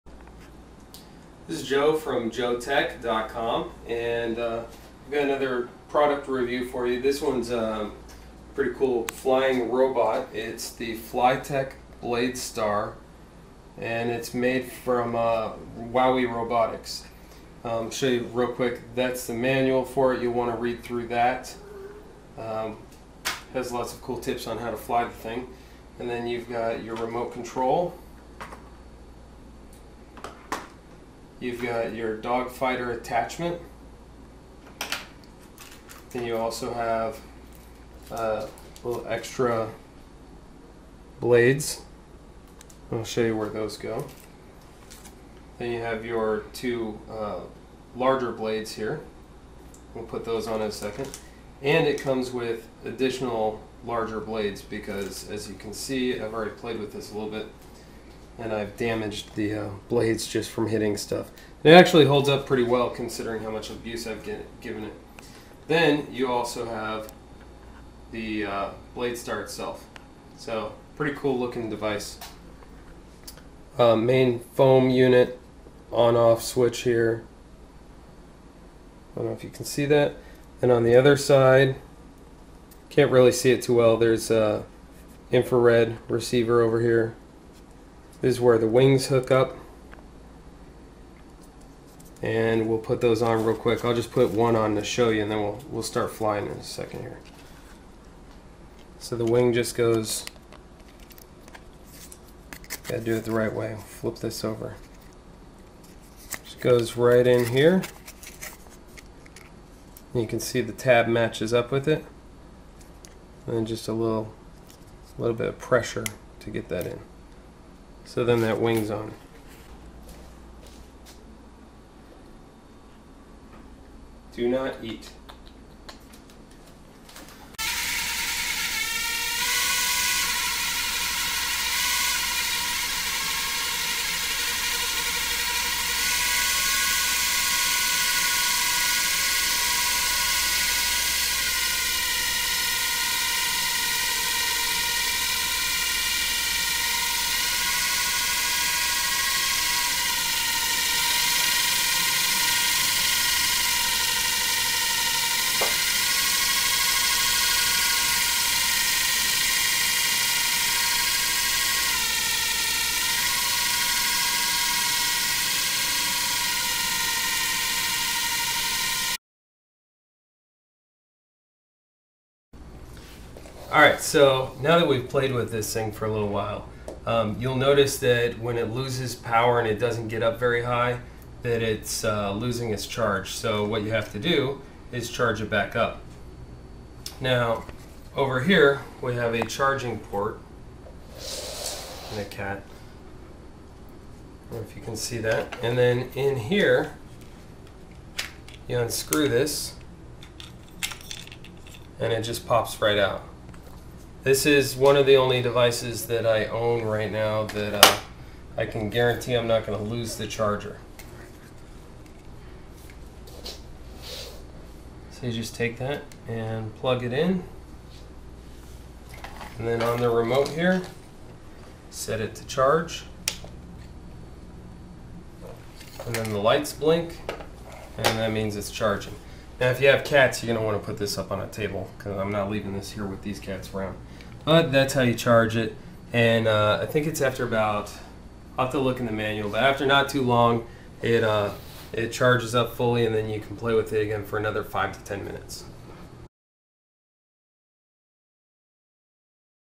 WowWee FlyTech Bladestar
This is the video portion of a review of WowWee's FlyTech Bladestar.  The full review can be seen on JoeTech.com tomorrow morning.